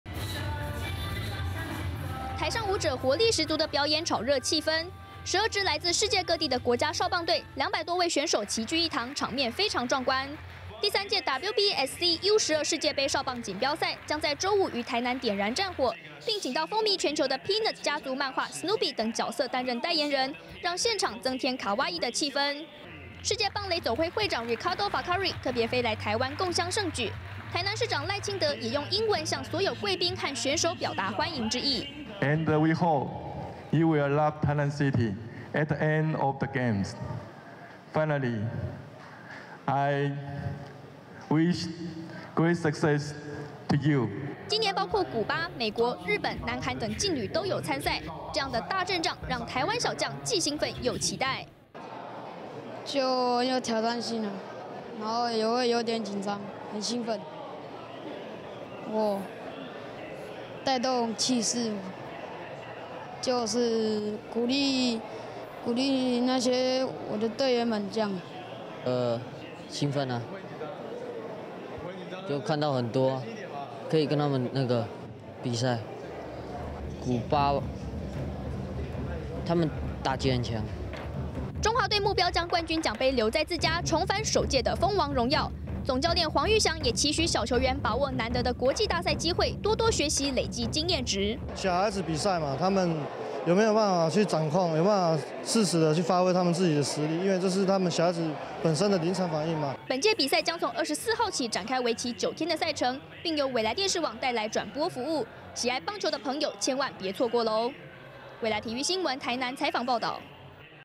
7/23U12世界盃少棒錦標賽 7/24引爆台南
第三屆WBSC U12世界盃少棒錦標賽將在週五於台南開打，今天12支國家少棒隊、共計兩百多位選手齊聚一堂參加賽前記者會，場面非常震撼，也讓中華小將對即將到來的比賽既興奮又期待。

這屆包括古巴、美國、日本、南韓等勁旅都有參賽，中華隊總教練黃郁翔認為，上了戰場最重要的是小球員要能處變不驚、拿出平常練習水準，也期許他們藉由這次大賽經驗累積經驗值。